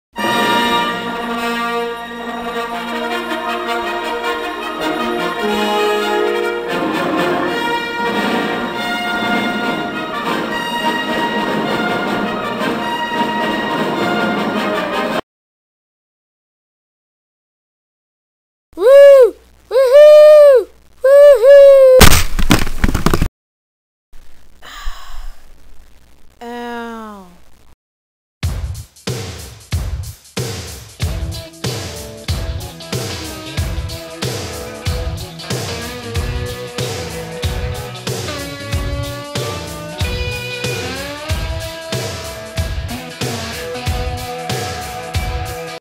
Lego Star Wars Lounge Special Short
Stan gets hit by a car